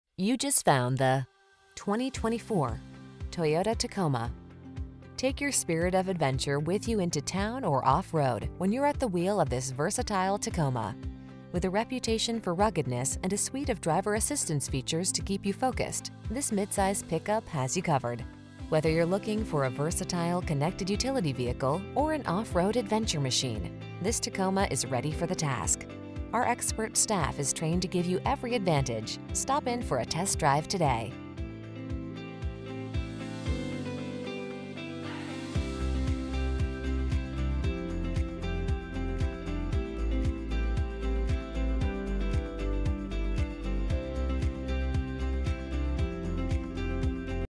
2024 Toyota Tacoma SR5 Hattiesburg, Jackson, Biloxi, Mobile, Sumrall MS
New 2024 Toyota Tacoma SR5 available at Toyota of Hattiesburg located in 6461 US Hwy 98, Hattiesburg, MS, 39402.

You just found the  2024  Toyota Tacoma. Take your spirit of adventure with you into town or off-road when you're at the wheel of this versatile Tacoma. With a reputation for ruggedness and a suite of driver assistance features to keep you focused, this mid-size pickup has you covered. Whether you're looking for a versatile, connected utility vehicle or an off-road adventure machine, this Tacoma is ready for the task. Our expert staff is trained to give you every advantage. Stop in for a test drive today!